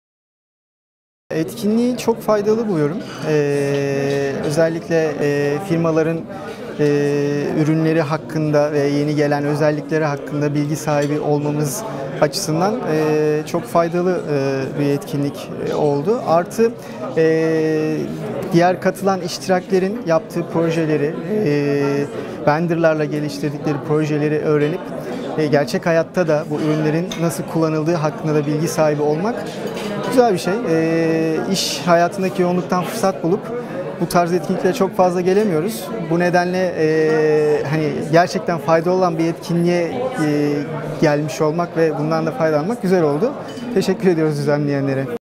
Axa Sigorta - Yaşar Geren Some ve Soc Etkinliği
SOME ve SOC etkinliğinde Axa Sigorta firmasından Yaşar Geren ile röportajımız